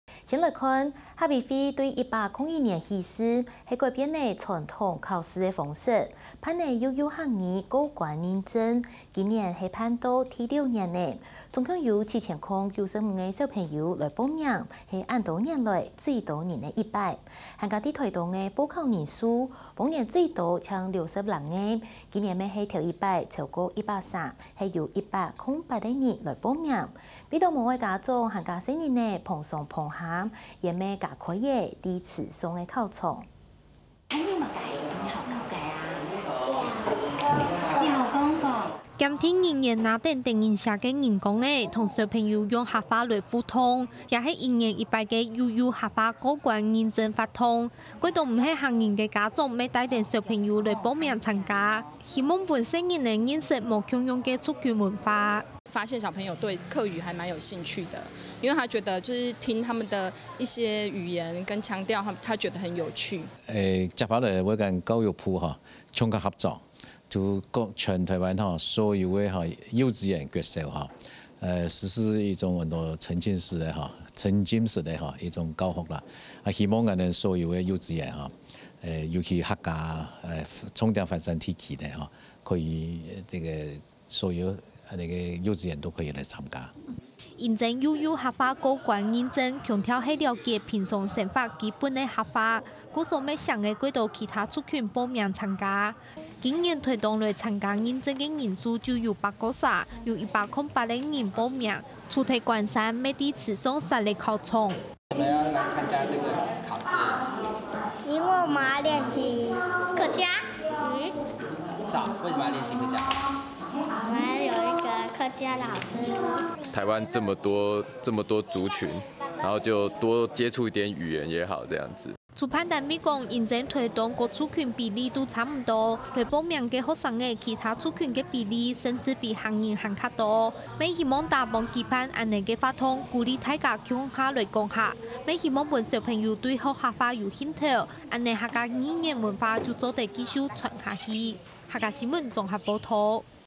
「幼幼客語闖通關」 逾7千人報名歷年之最【客家新聞20170520】
客委會從101年開始，突破傳統考試的模式，舉辦「幼幼客語闖通關認證」，今年也已經邁入第6年，總共有7095位小朋友報名，是歷年來最多人的一次，而台東報考人數，往年最多僅有60多位，今年也首次突破百人，有108人報名，為了減少家長學童奔波，也增設了池上考場。